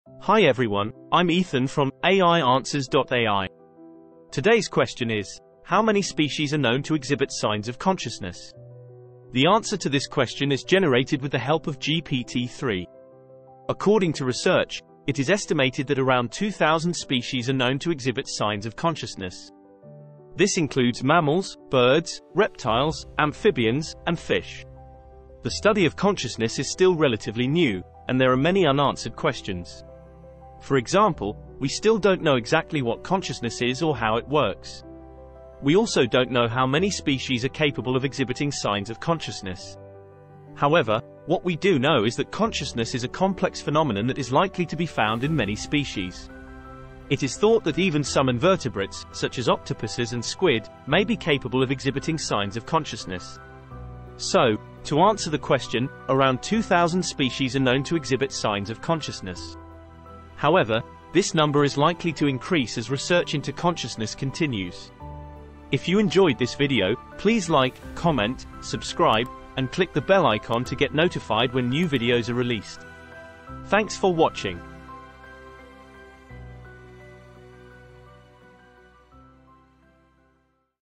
GPT-3 Answers: How Many Species Show Signs of Consciousness?
Today we ask GPT3: How many species are known to exhibit signs of consciousness?

GPT3 responds:

There is no definitive answer to this question, as it is not yet known how to accurately measure consciousness in other species. However, some species are thought to exhibit signs of consciousness, including primates, dolphins, elephants, and cetaceans. Additionally, some researchers have suggested that certain species of birds, reptiles, and fish may also exhibit signs of consciousness.